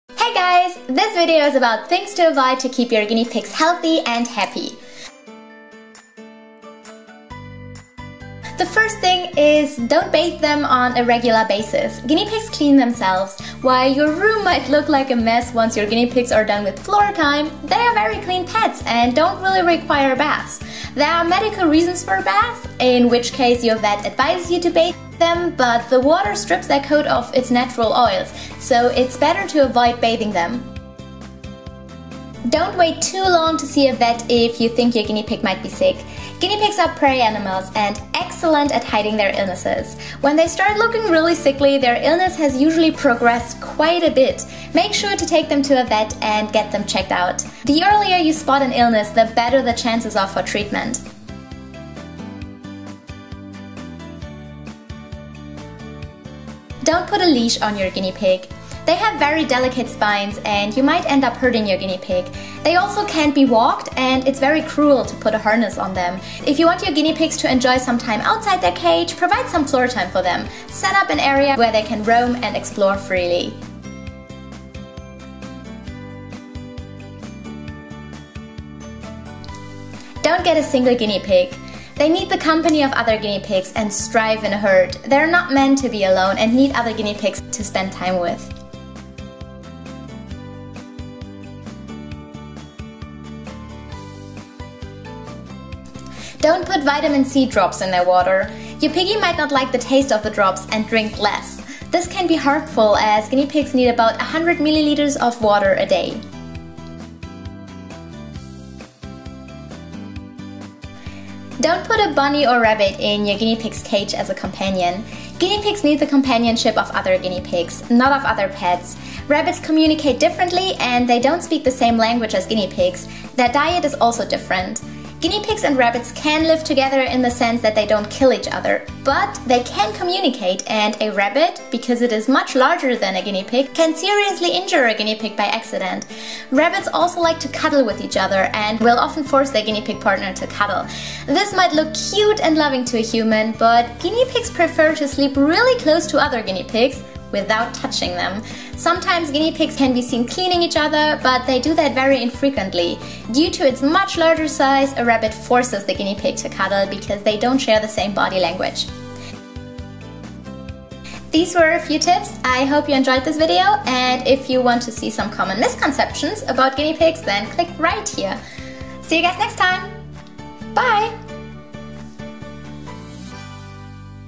Things to Avoid to Keep Your Guinea Pigs Healthy
Things to avoid to keep your guinea pigs happy and healthy :)